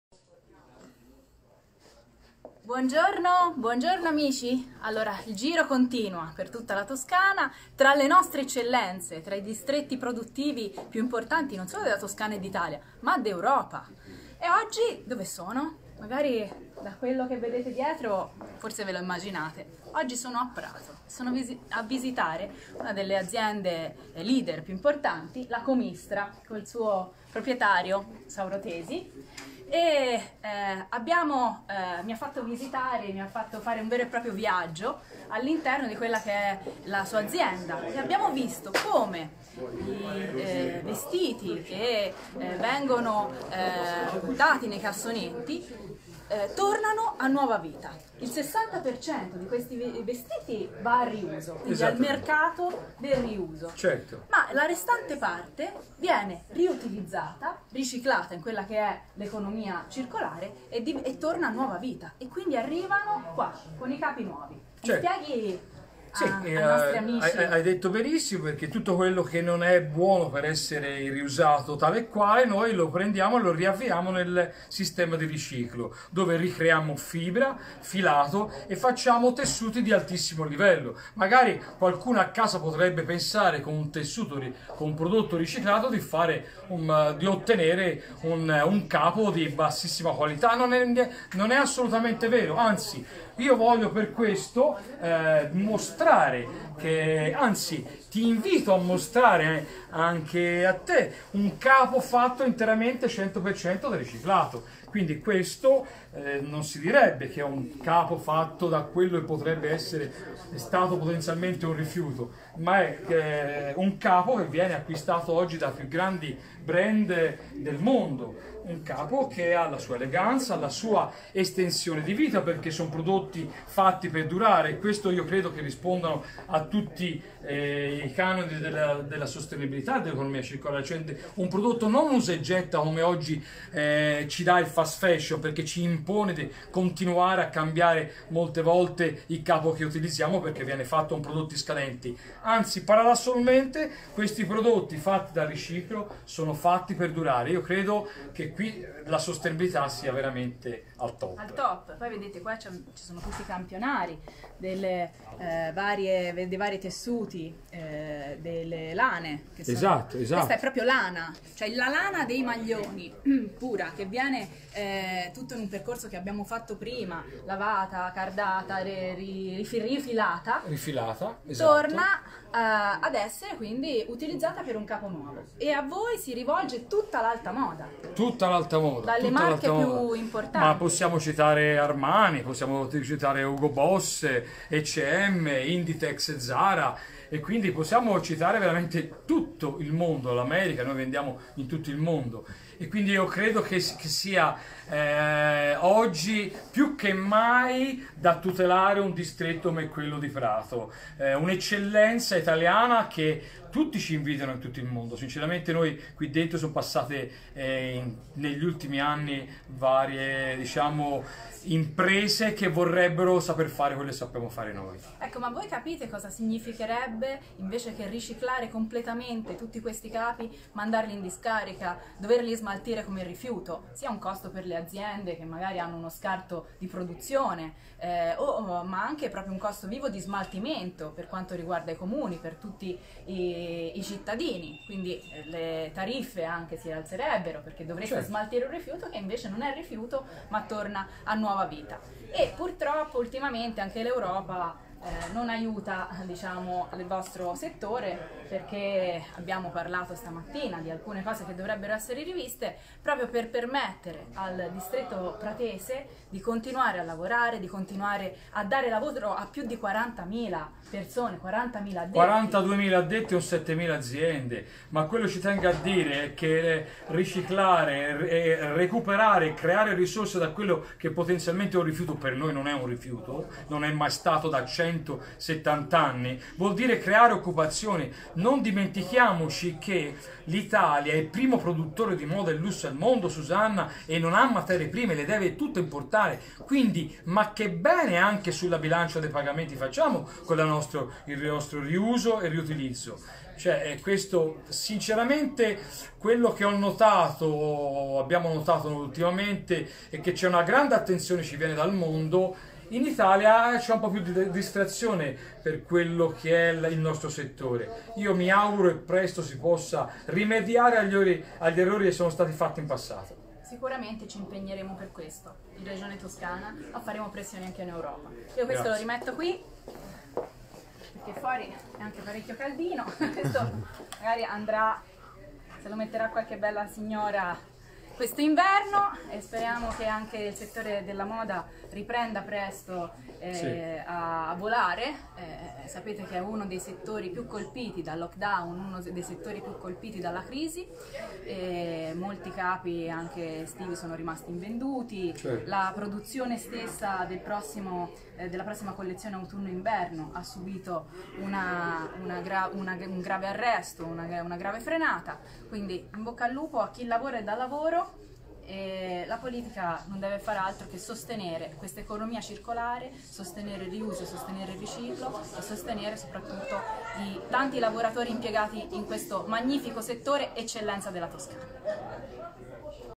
PRATO, DOVE GLI SCARTI DIVENTANO PRODOTTI DI ALTA MODA - SUSANNA CECCARDI
(Agenzia Italia News) PRATO, DOVE GLI SCARTI DIVENTANO PRODOTTI DI ALTA MODA - SUSANNA CECCARDI